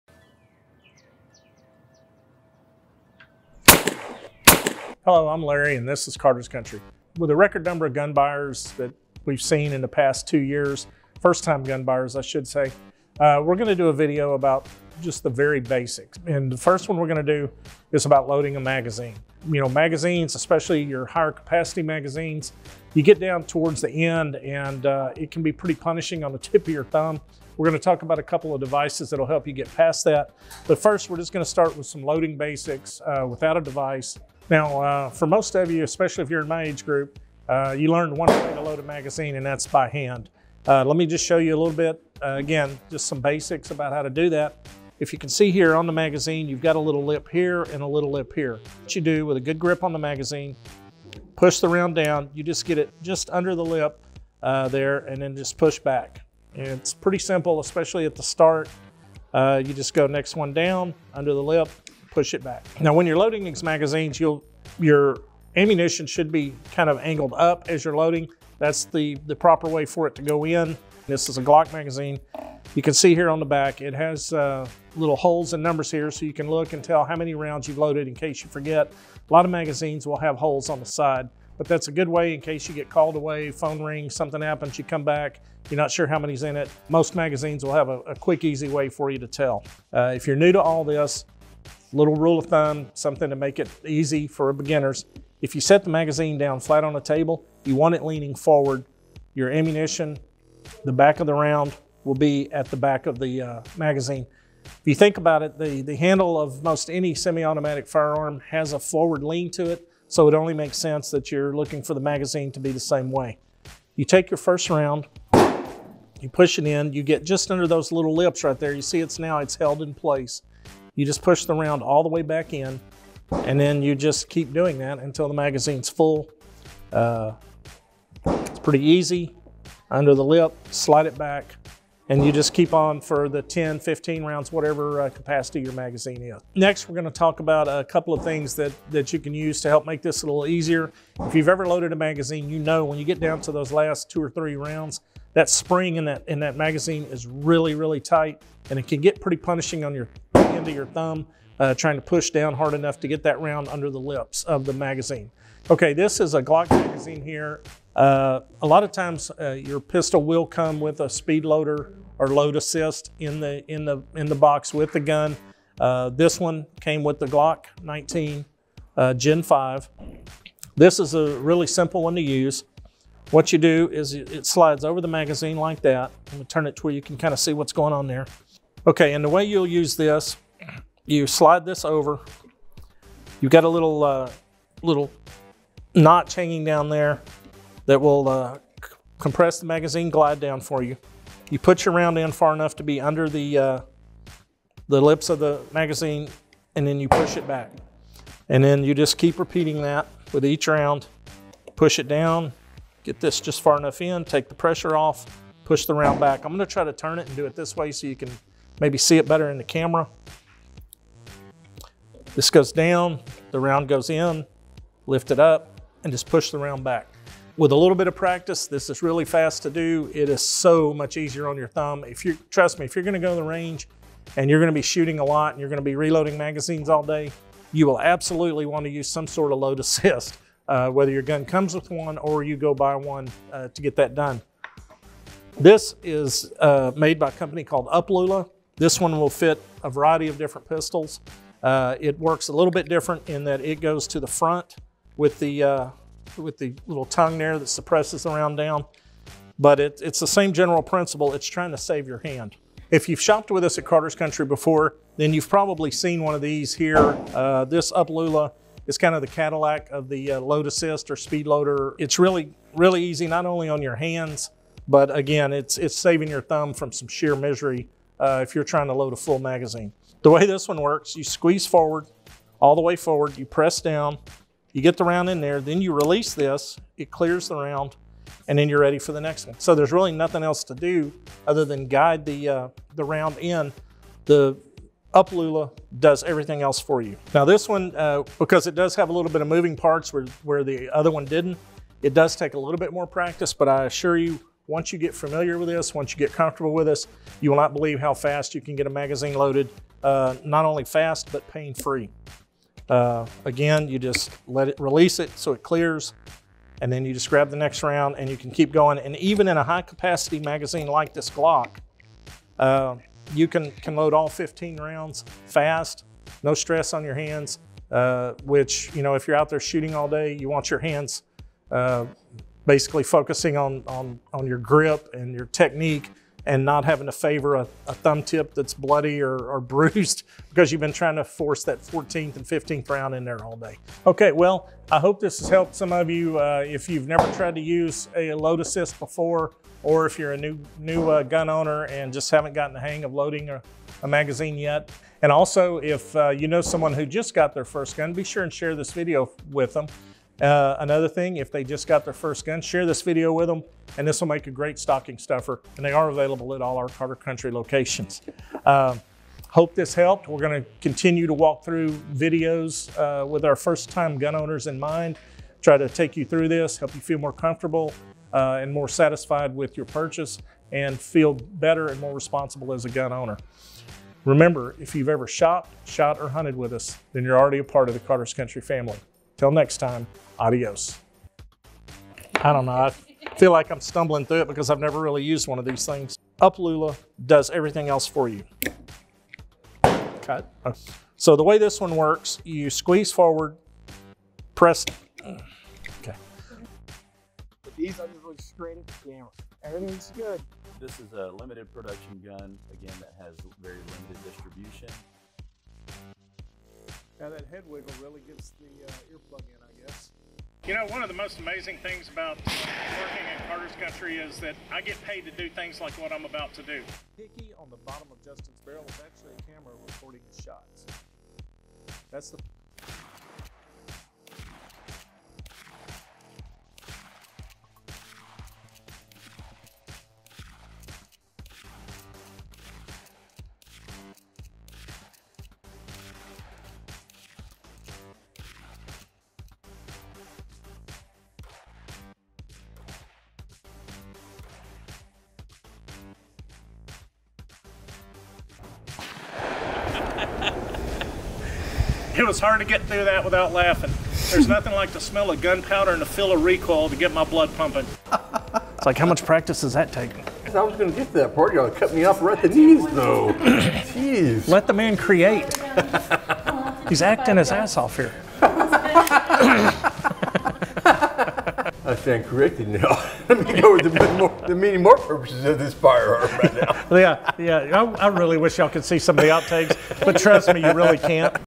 How to load a gun magazine EASY, for beginners!
We're going back to basics with this one! Larry goes over how to load a magazine and know you've done it right!